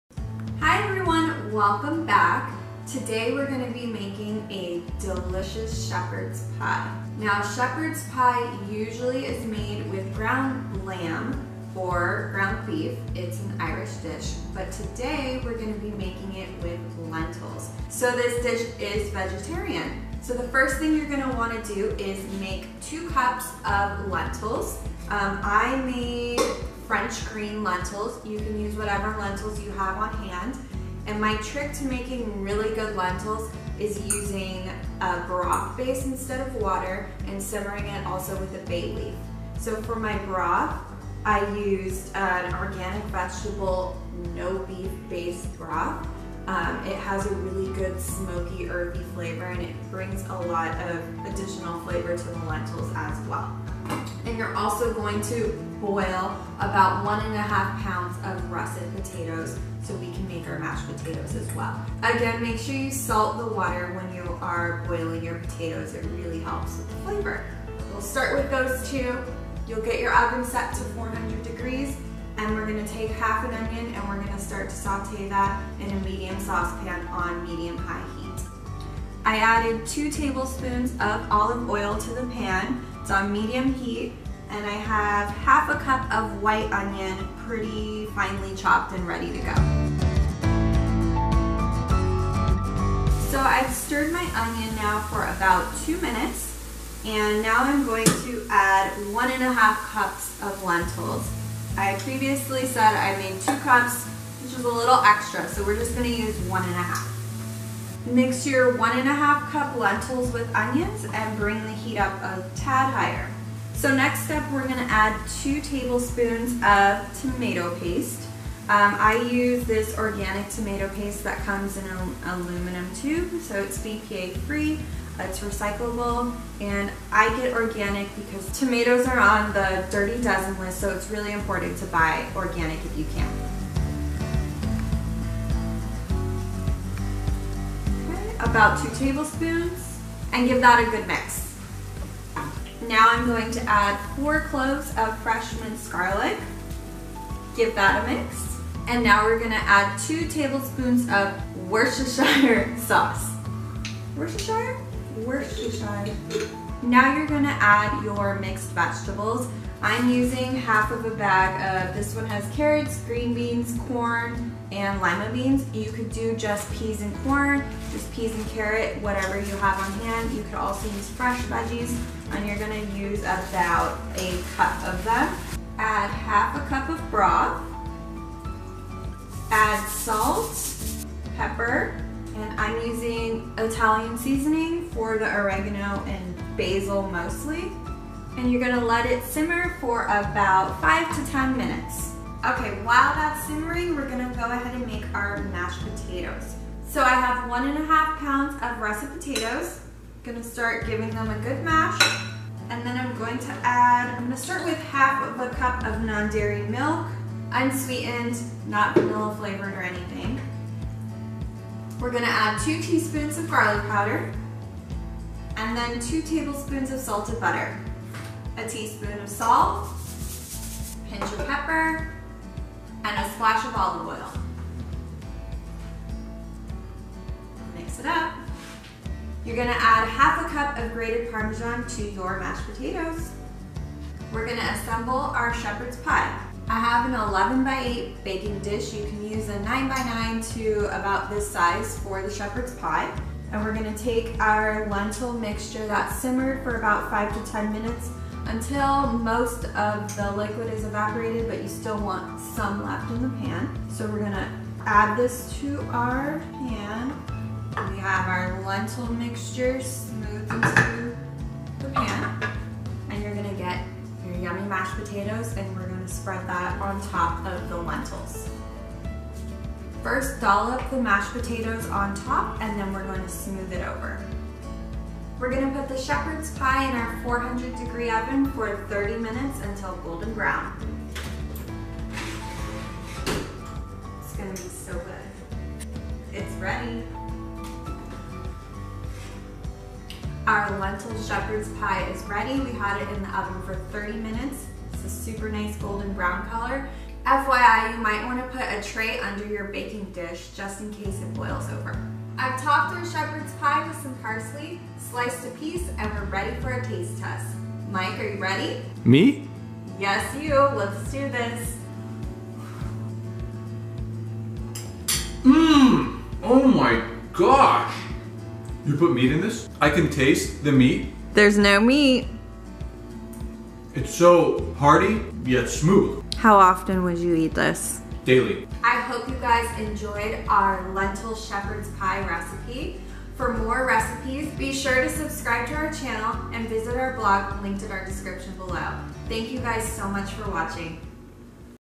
Lentil Shepherd's Pie Recipe | Vegetarian
Hello, and welcome to Nest & Nature! In this video, we can't wait to share our Vegetarian Lentil Shepherd's Pie recipe with you!

Favorite Baking Dishes
Makes the best vegetarian broth
Delicious French Green Lentils

This is such an incredible dinner recipe because it tastes so good. The icing on the cake is how healthy, natural, and easy this recipe is; best of all, it is vegetarian, and while our variation is not vegan, making it a vegan recipe is very easy to do.

Our shepherd's pie offers the delicious flavor and texture of lentils, and also includes onion, garlic, mixed veggies, olive oil, potatoes, and other tasty ingredients. Even if you're not a vegetarian, you're sure to enjoy this.

We are especially excited about the broth we use in our recipe. It contains no meat and is completely vegetarian, and at the same time, it tastes so good.

Our YouTube channel is supported by amazing viewers like YOU!

We hope you enjoy this as much as we enjoyed making it!

- Mike & Aysha